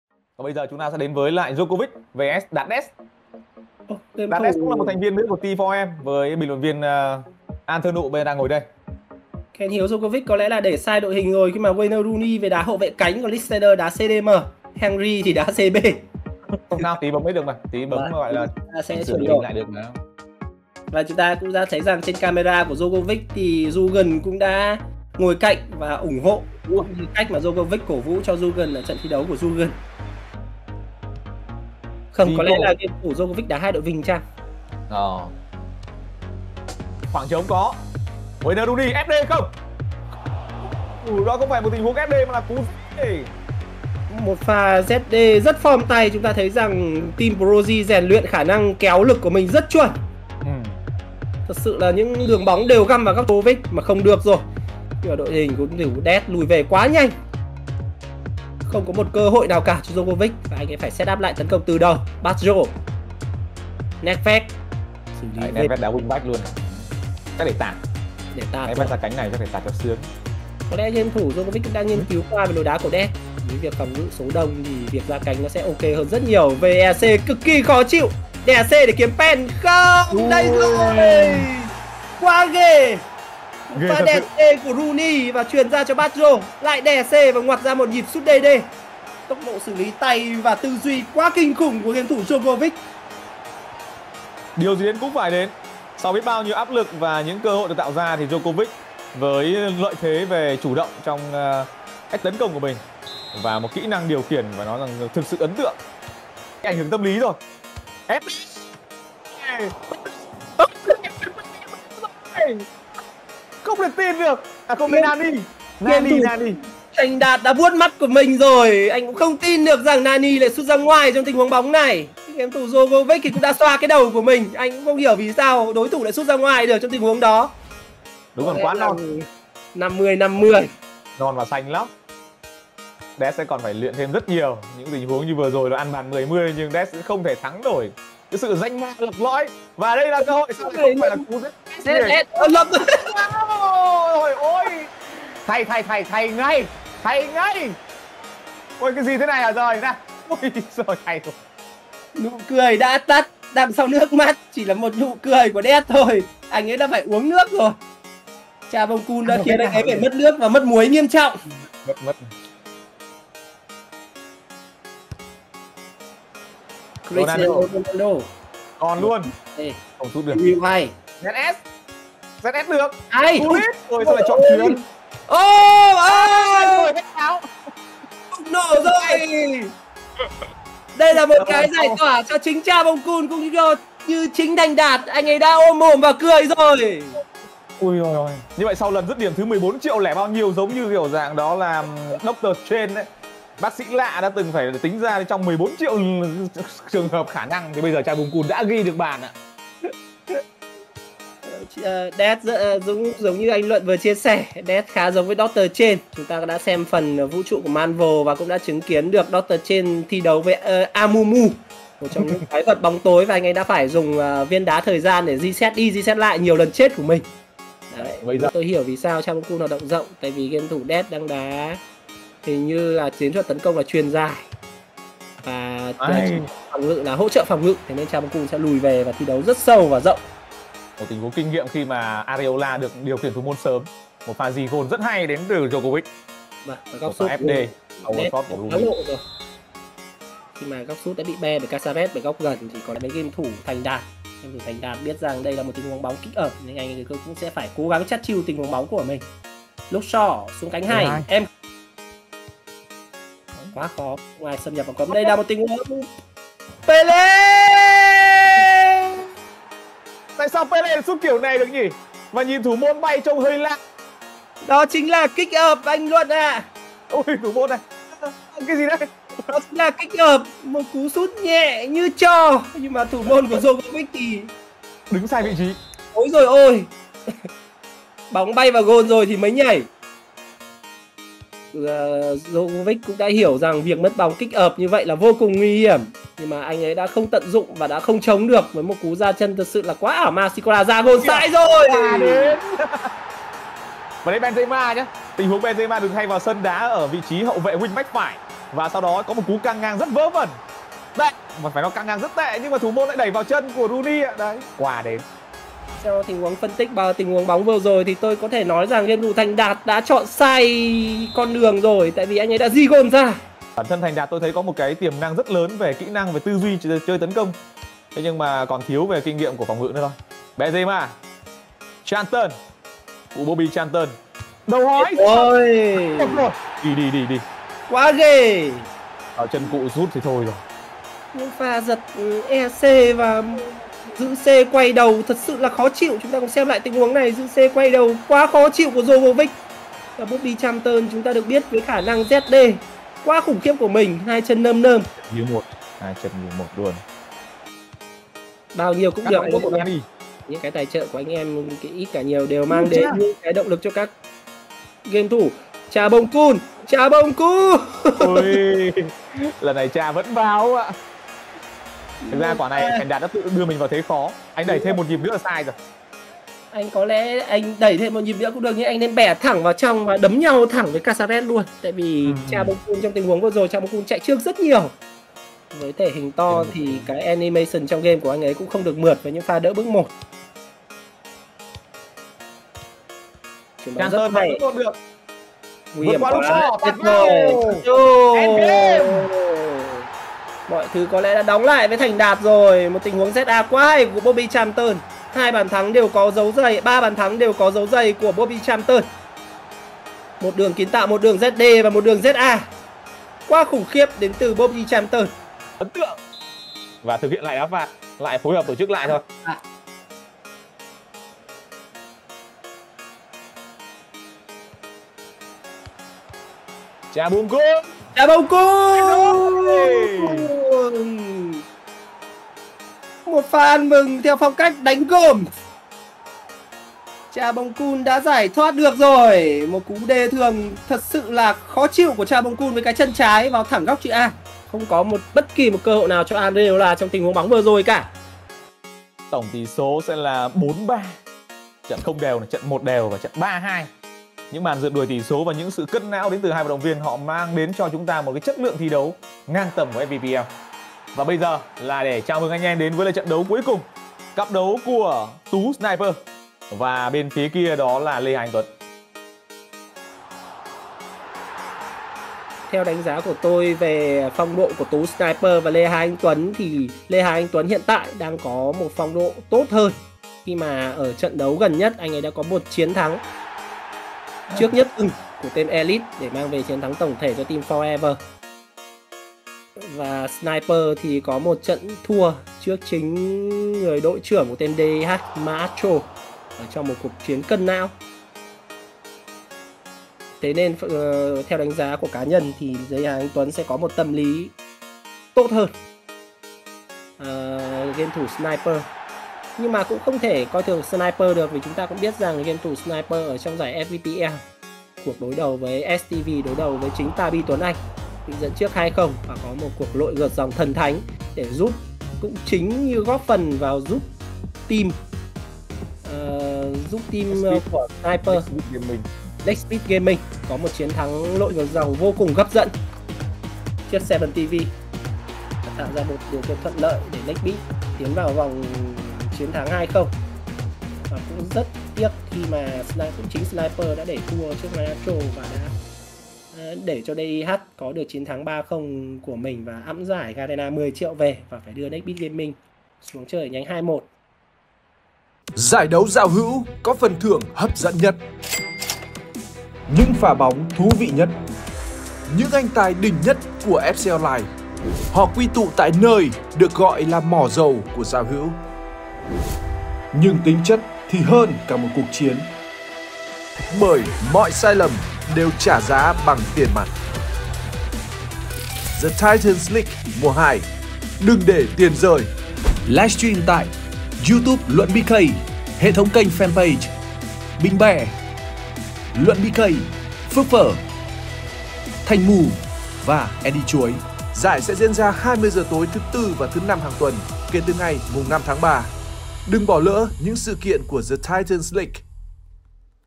Thành Đạt suýt gây khó dễ, Djokovic vất vả đi tiếp | THE TITANS LEAGUE SEASON 2
© Bản quyền thuộc về BÌNH BE, vui lòng không reup dưới mọi hình thức
© Copyright by BÌNH BE ☞ Do not Reup